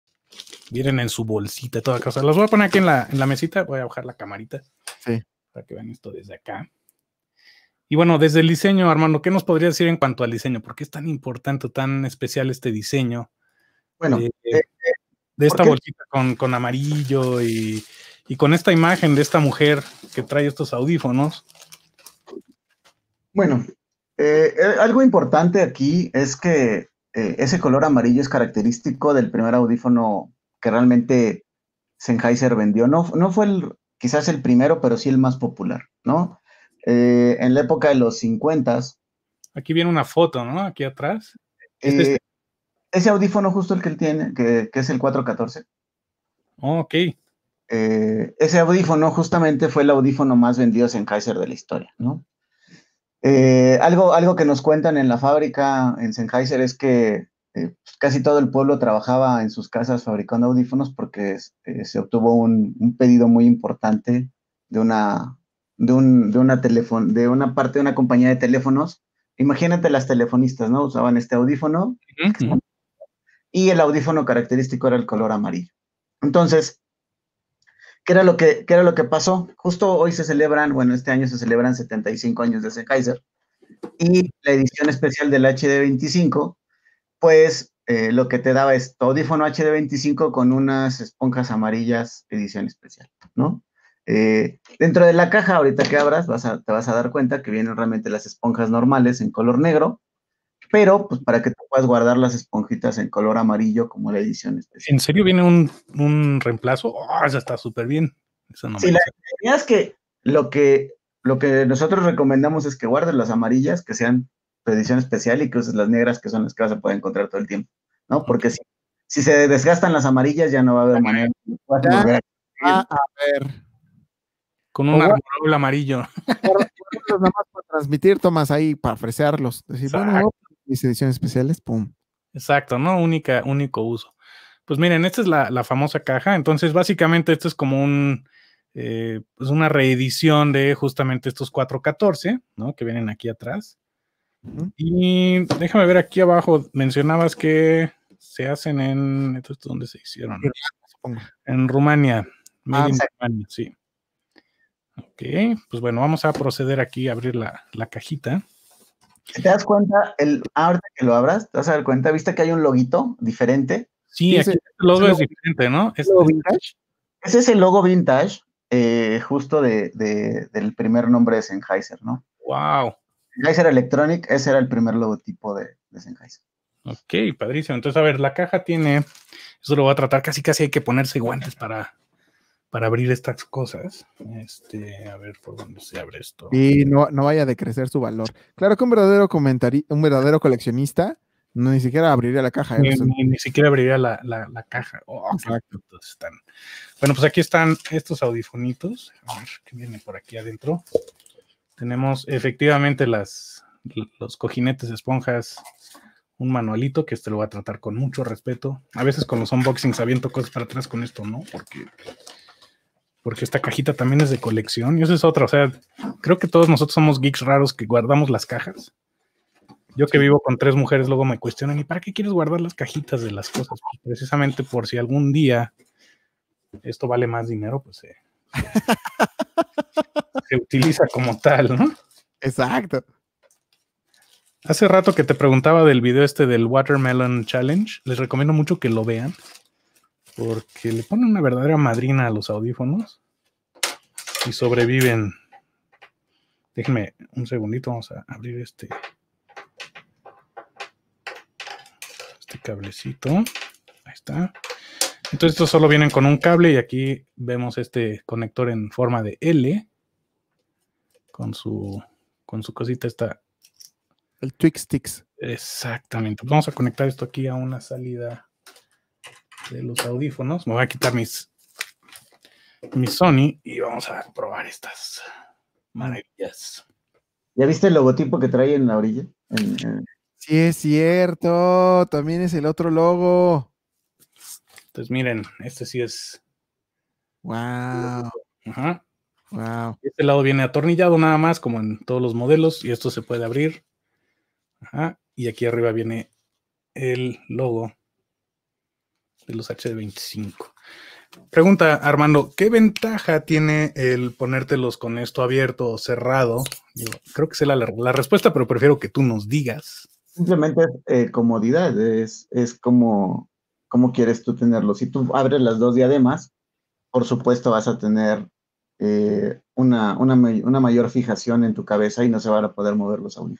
Sennheiser HD25 Limited Edition - Unboxing
Una muestra del video completo en el que hablamos sobre los fenomenales y legendarios audífonos HD25 de Sennheiser.

Recomendaciones de productos